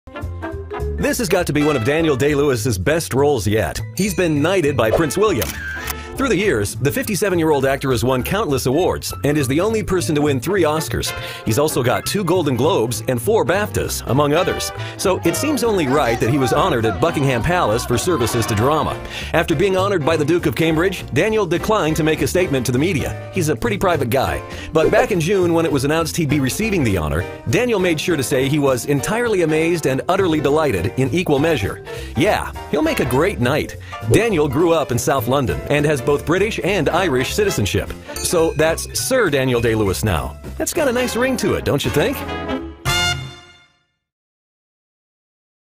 Sir Daniel Day-Lewis Has Been Knighted By Prince Wil...
The 57-year-old actor Daniel Day-Lewis has been made a knight by Prince William. The actor has appeared in countless award-winning films like Gangs of New York, Lincoln, There Will Be Blood and My Left Foot. He was honored at Buckingham Paliace for services to drama.